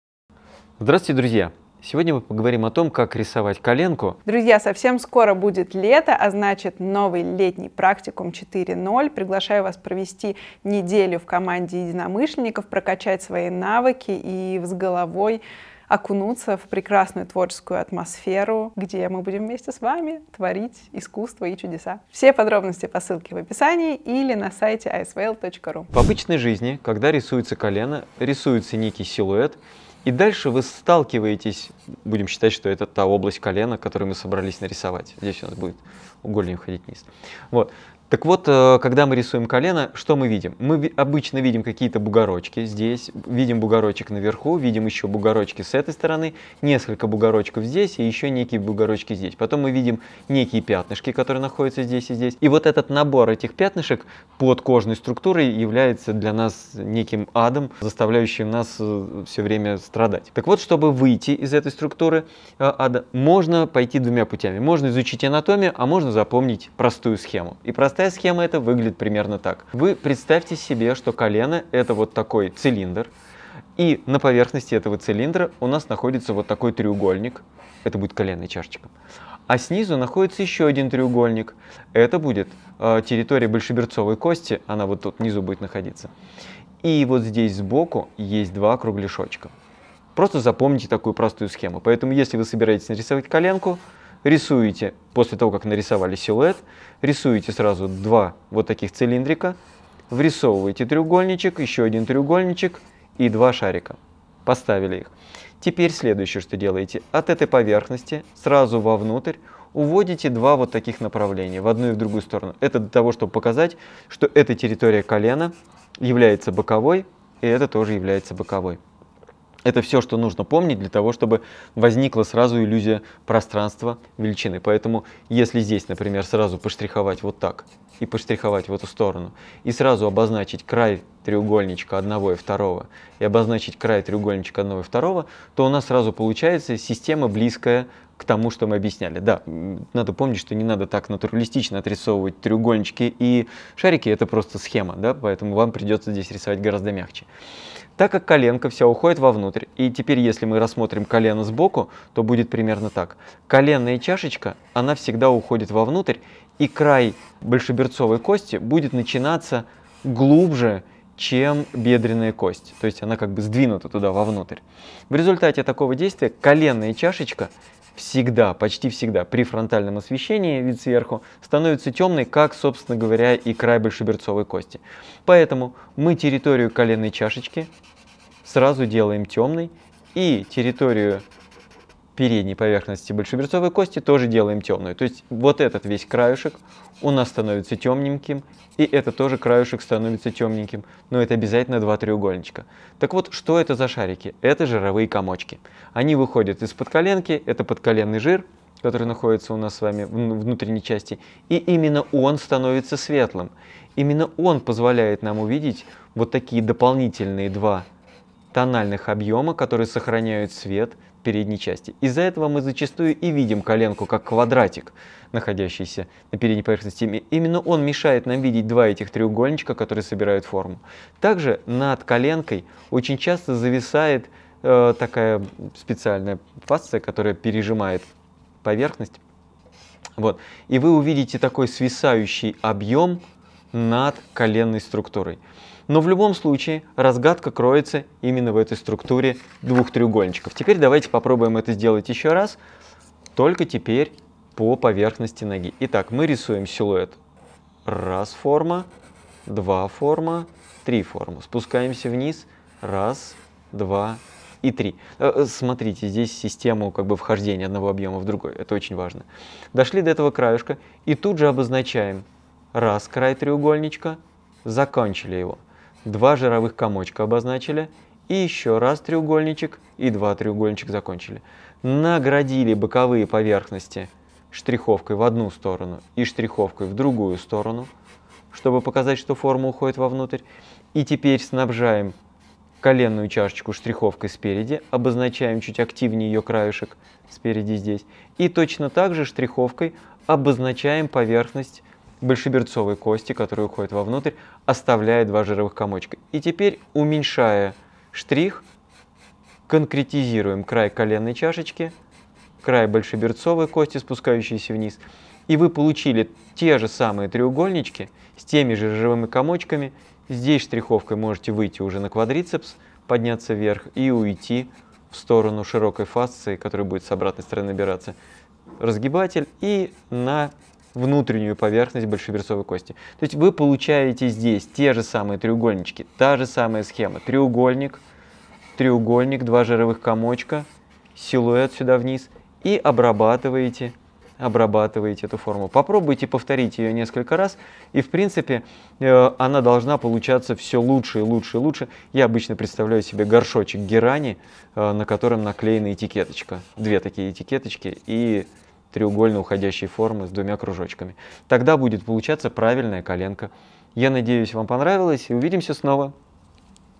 Как обычно рисуют КОЛЕНО и как правильно рисовать!? - Александр Рыжкин
😍 Летний практикум "Как рисовать" 4.0 (30 июля - 5 августа 2023 года)